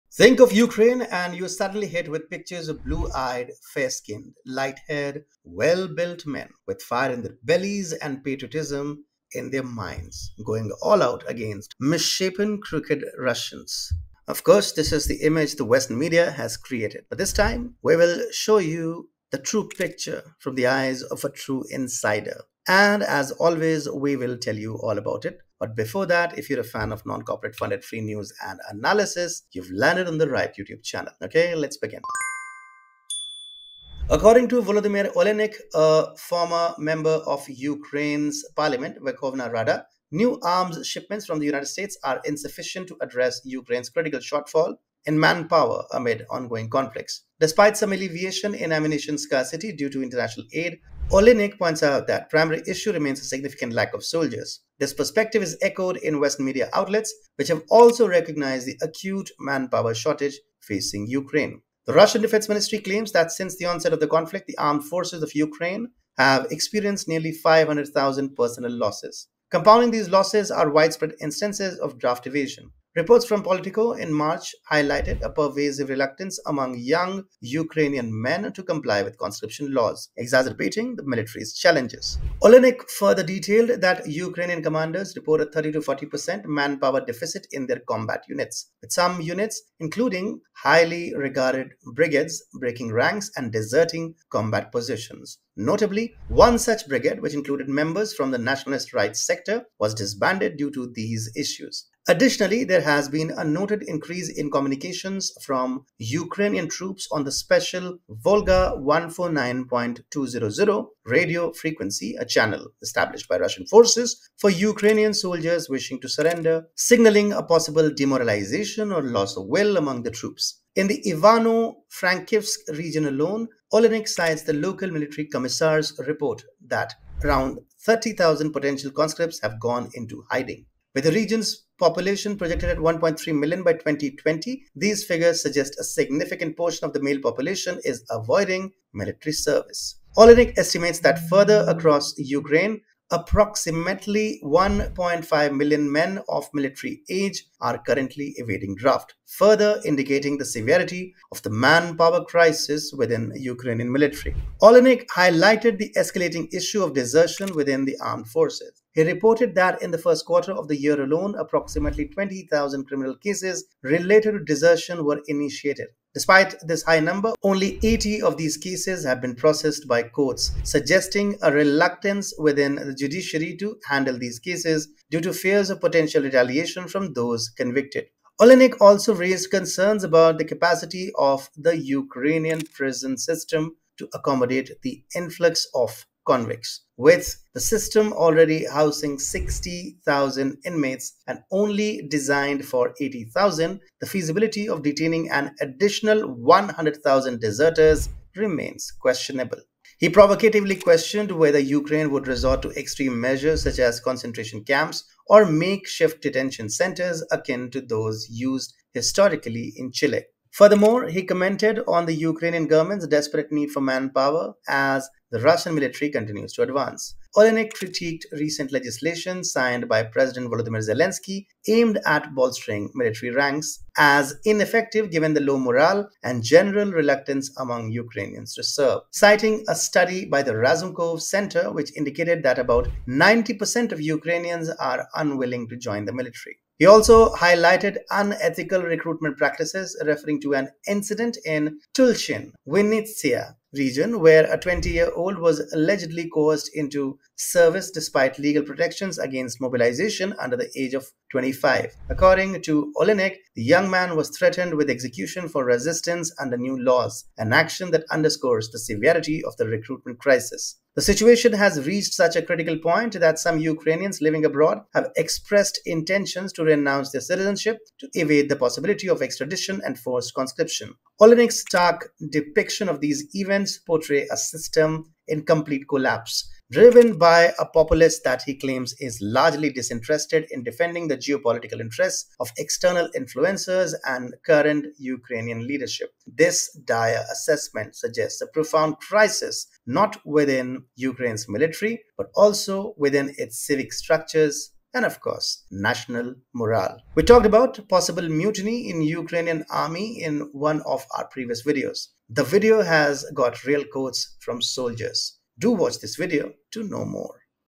Former Ukrainian MP exposes Ukraine’s REAL CRISIS
Oleynyk further detailed that Ukrainian commanders report a 30-40% manpower deficit in their combat units, with some units, including highly regarded brigades, breaking ranks and deserting combat positions. Notably, one such brigade, which included members from the nationalist Right Sector, was disbanded due to these issues.

Additionally, there has been a noted increase in communications from Ukrainian troops on the special "Volga" 149.200 radio frequency—a channel established by Russian forces for Ukrainian soldiers wishing to surrender—signaling a possible demoralization or loss of will among the troops.